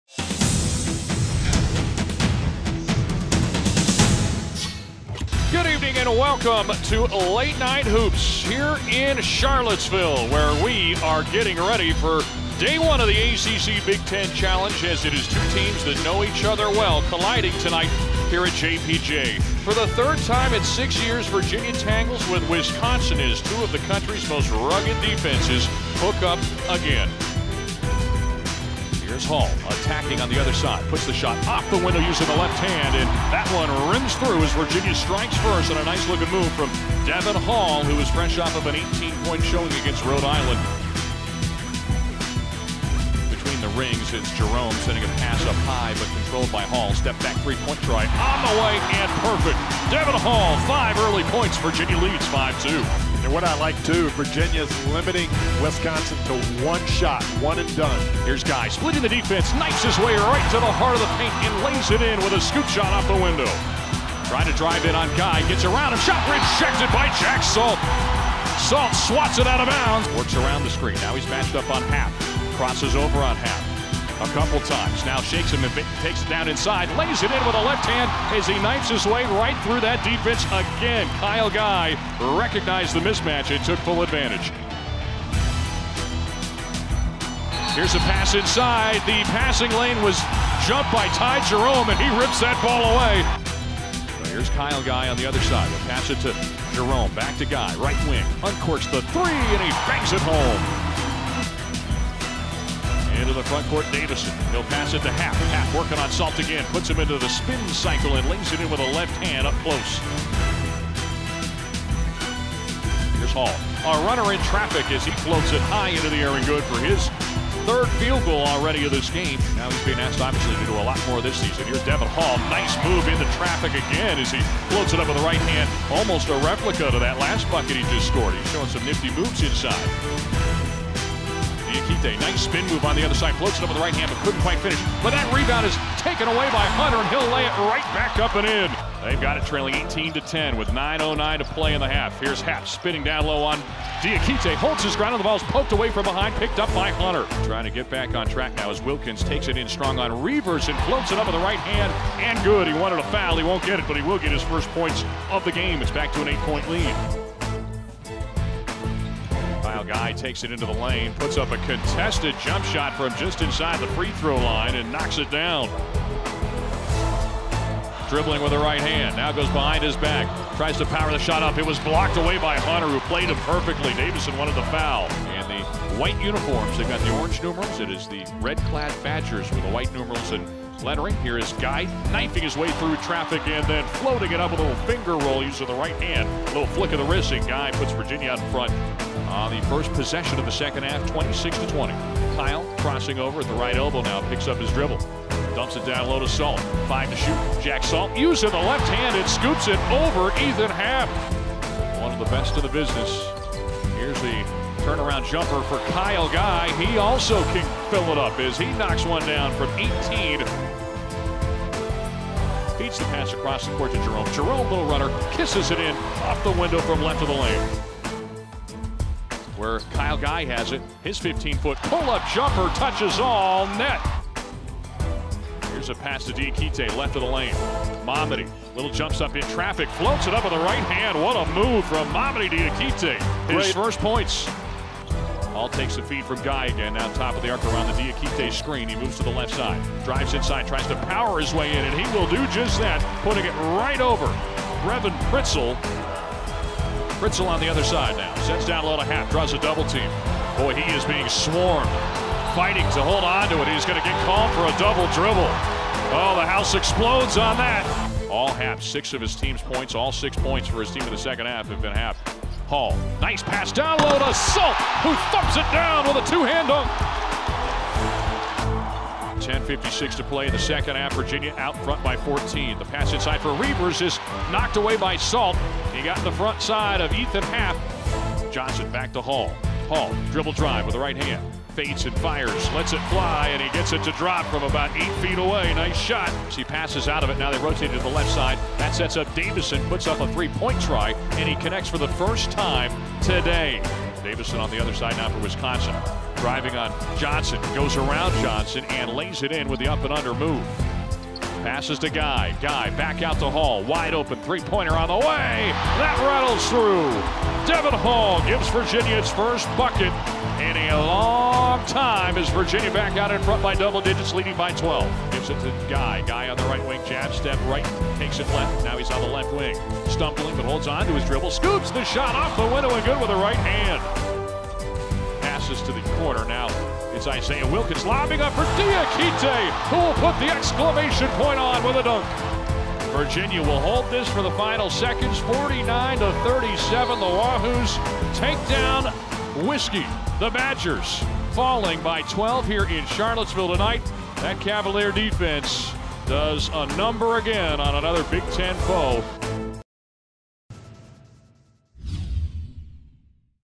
MEN'S BASKETBALL - Virginia vs. Wisconsin Highlights
The No. 18 Cavaliers clamp down defensively and claim their fourth straight win in the Big Ten/ACC Challenge.